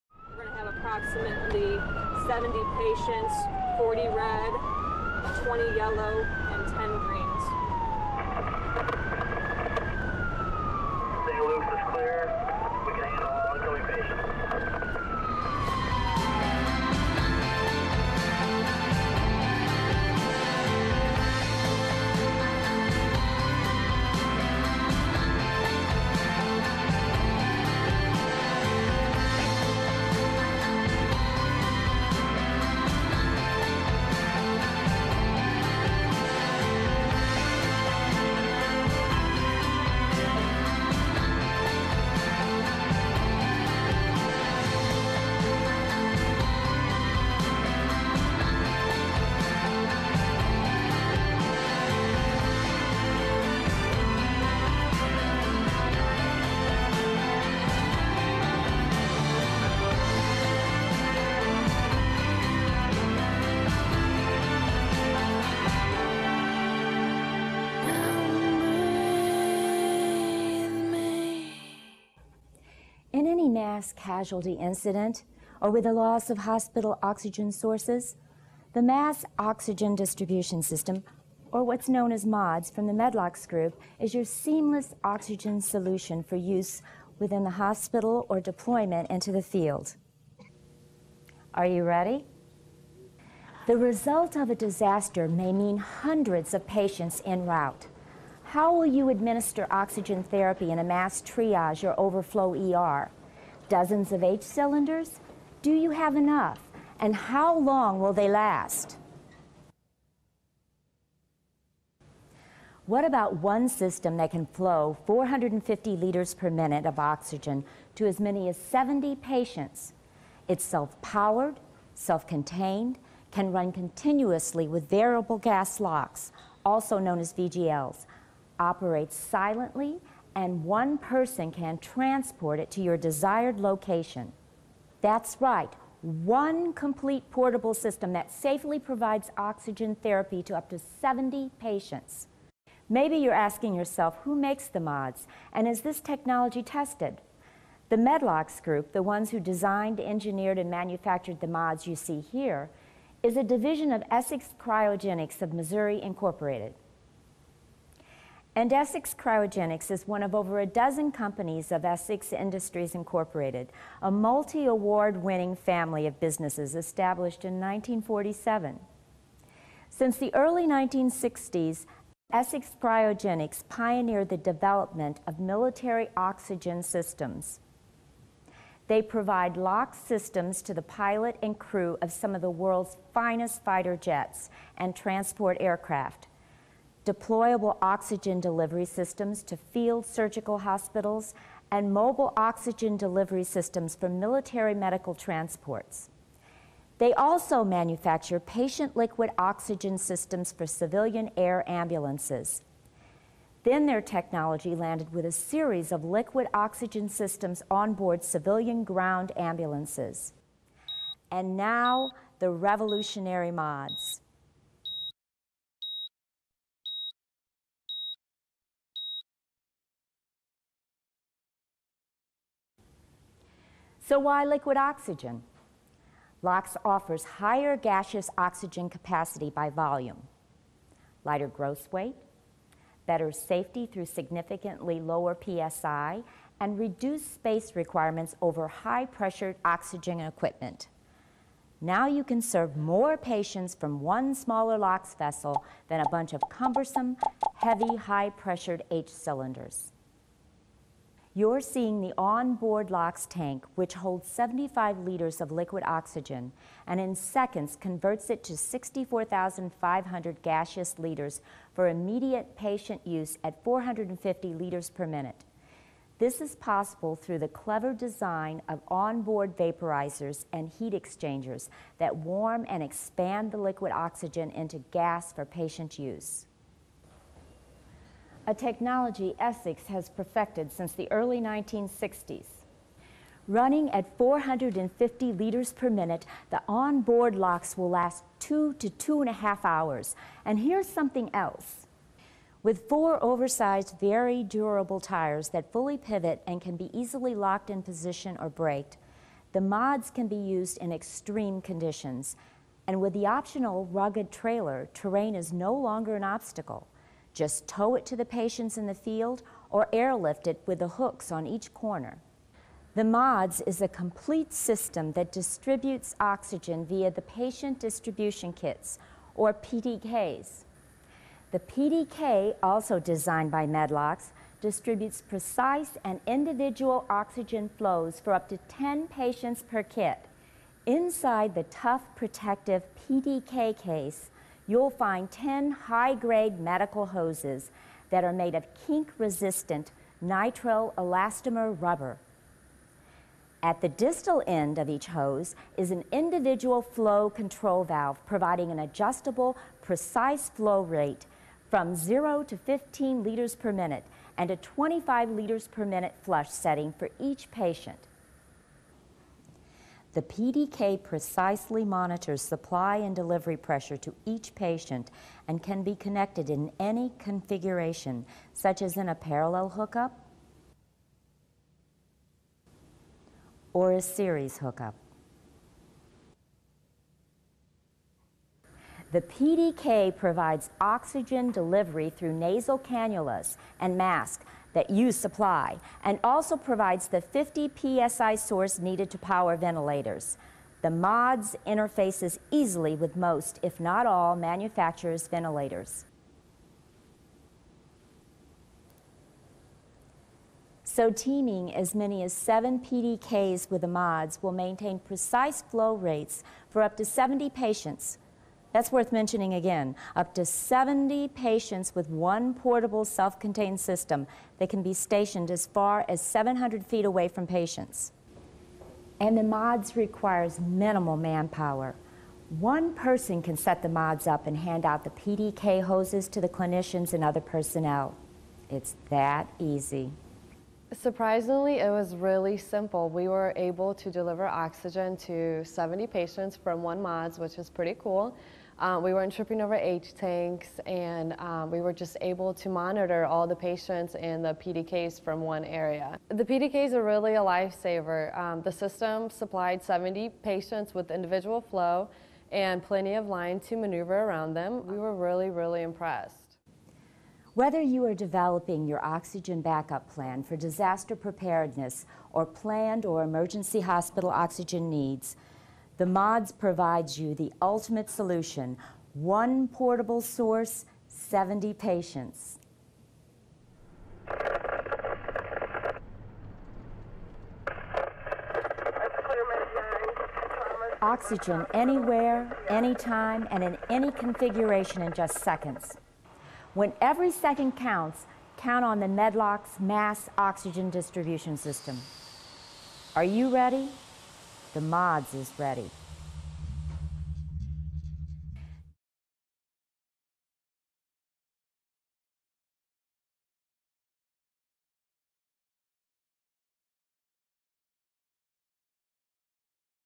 Penn Care - Mass Oxygen Distribution System for Mass Casualty Incident
In an accident, every second counts. With MODS you can rest easy as it provides seamless oxygen solution to patients in the event of a mass casualty incident.

What are the benefits of MODS?
•Can supply 450 LPM to up to 70 patients
•No need for external power source, completely portable and self contained.
•Provides precise, adjustable and individual oxygen flow to each patient
•Can be deployed to the field with the help of MCI Trailers
•MCI Trailers can be custom built to meet specific requirements